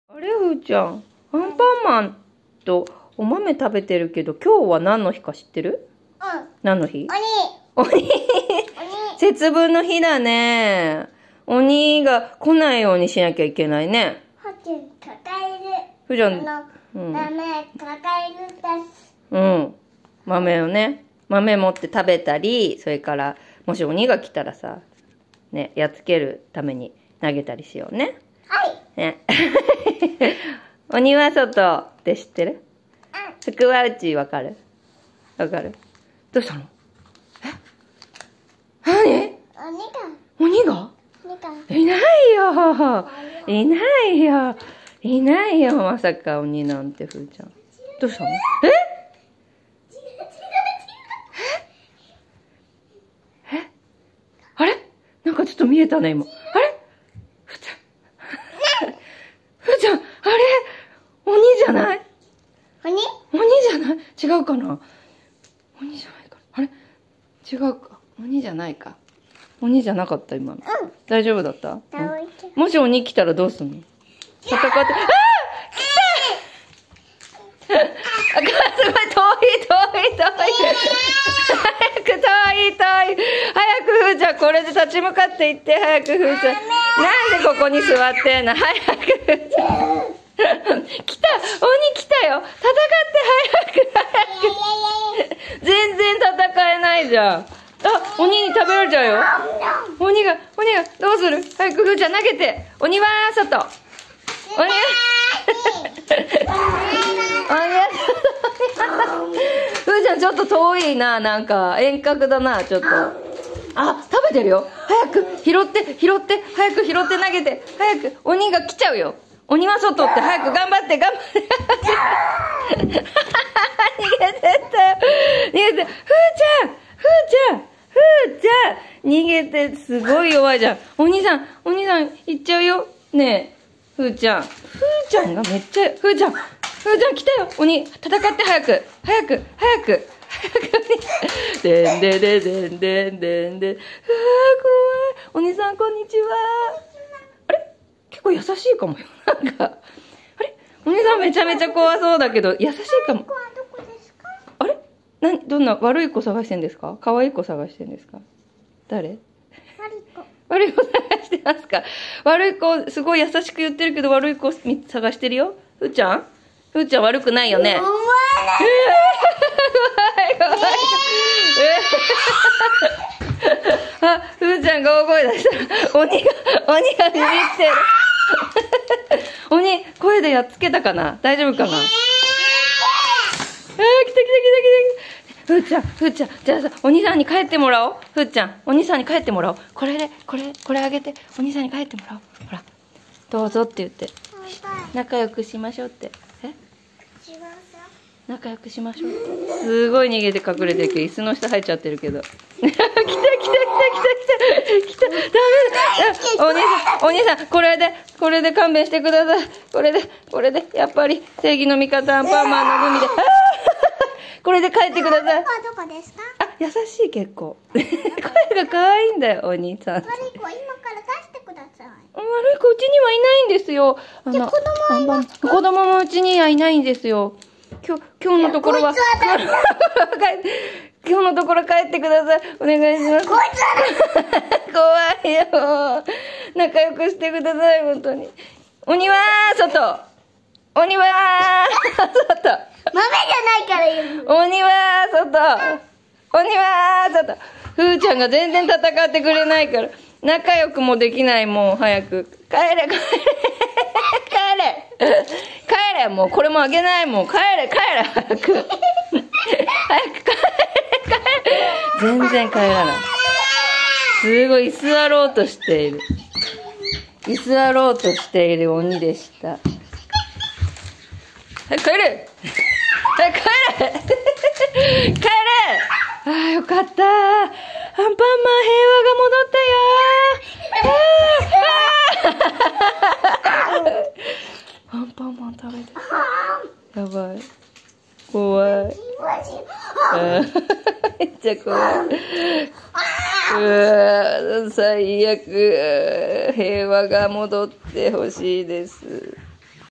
【節分の日のおウチ遊び】節分
節分なので、お姉ちゃんが幼稚園で鬼のお面を作ってきました👹✨
…ということで！！鬼ごっこ⁉鬼退治ごっこ⁉🤭
で、おウチ遊びを全力で楽しもう🥳🥳🎉